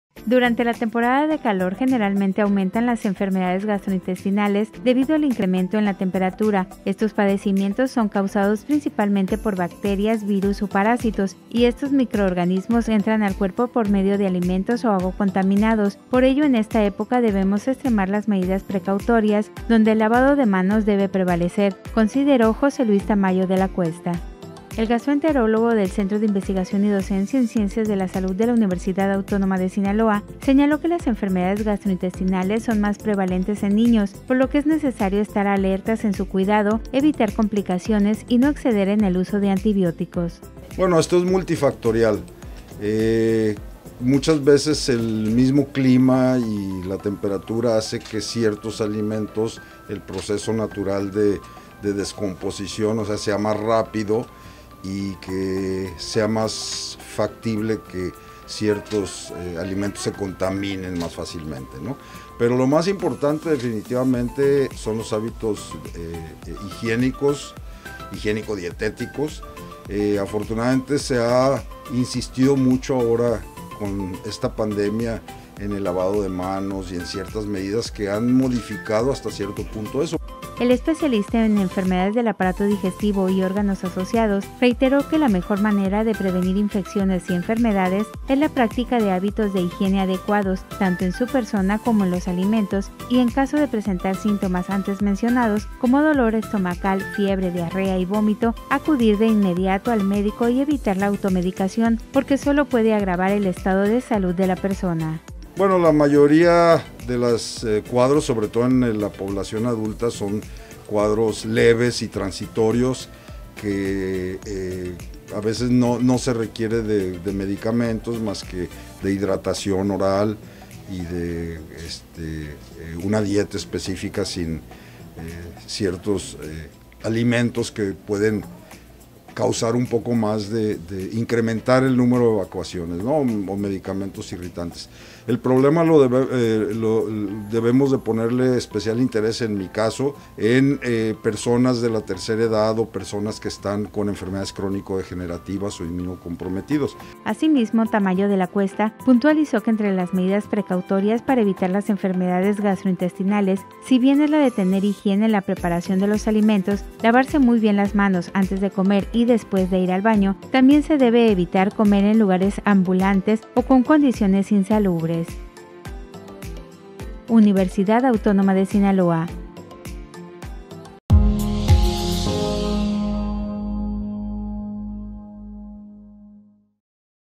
Temporada de calor detona las enfermedades gastrointestinales
Durante la temporada de calor generalmente aumentan las enfermedades gastrointestinales debido al incremento en la temperatura, estos padecimientos son causados principalmente por bacterias, virus o parásitos, y estos microorganismos entran al cuerpo por medio de alimentos o agua contaminados, por ello en esta época debemos extremar las medidas precautorias, donde el lavado de manos debe prevalecer, consideró José Luis Tamayo de la Cuesta.